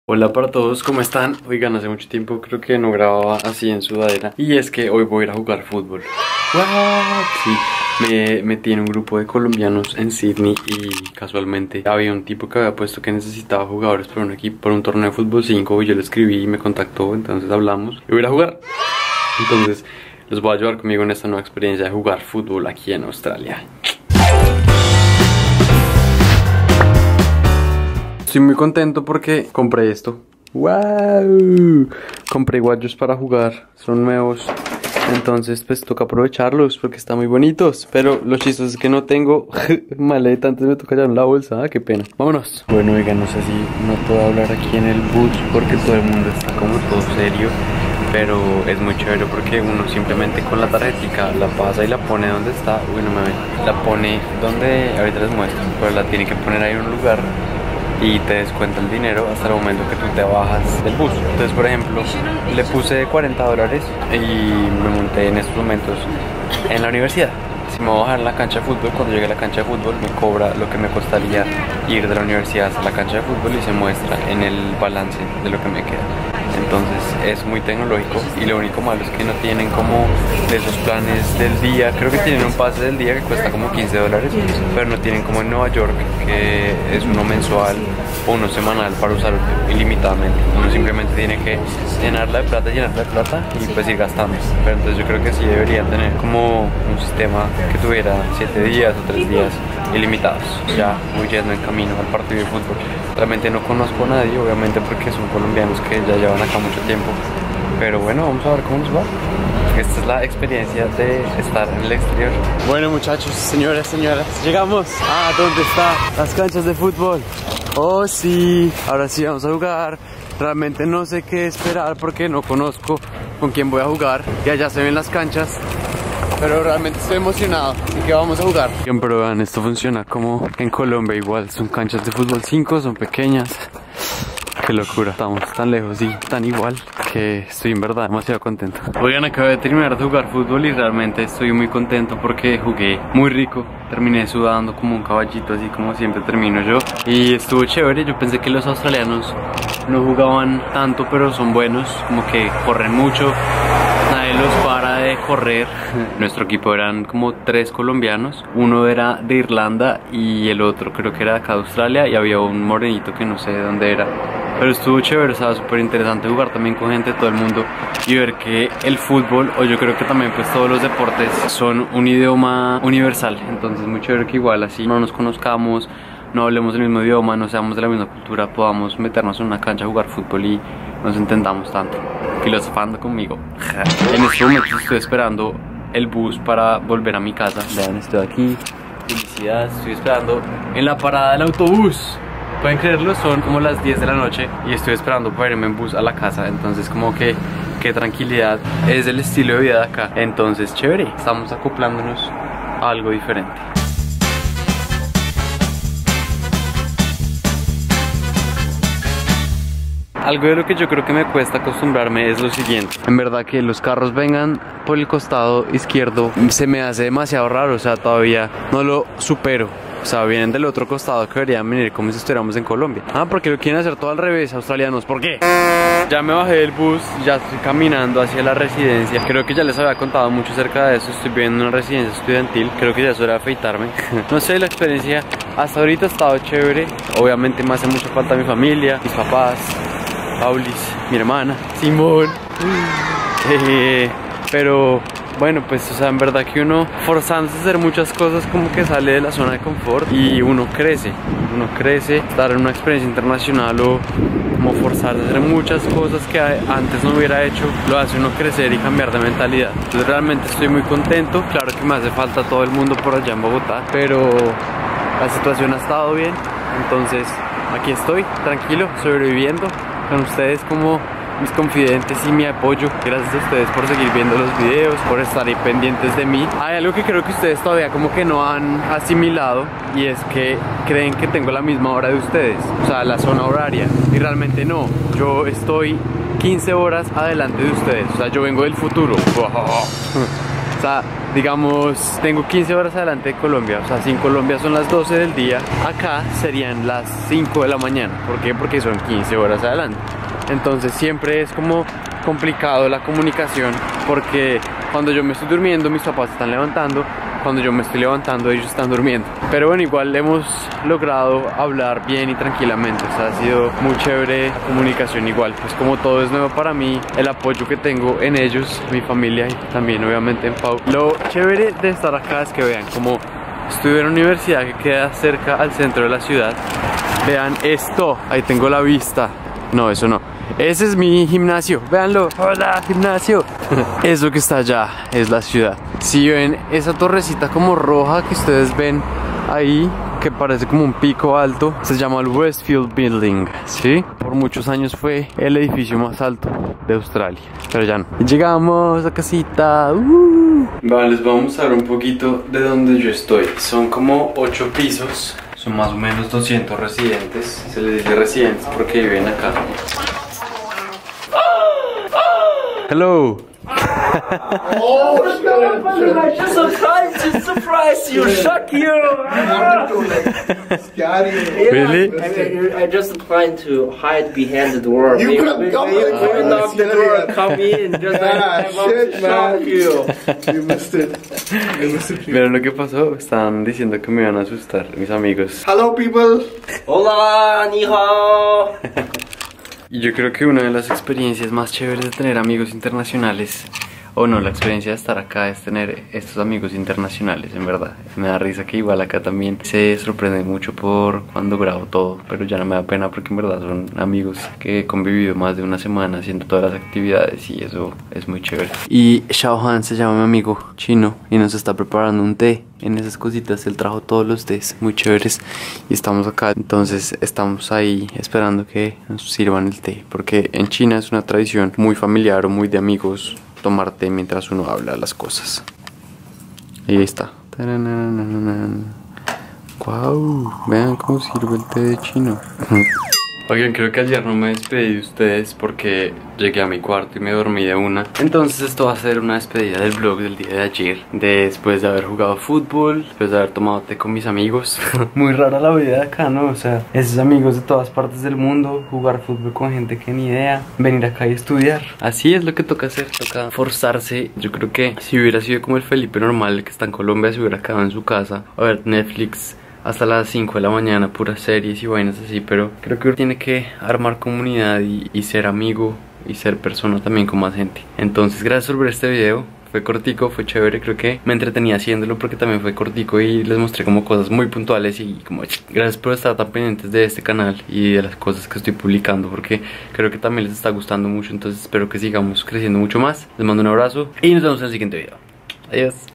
FÚTBOL EN AUSTRALIA? :D - FELIPE ZULUAGA
En este video fui muy feliz. Pude volver a jugar fútbol como lo hacía en Colombia. Poquito a poco se van acomodando todas las cosas.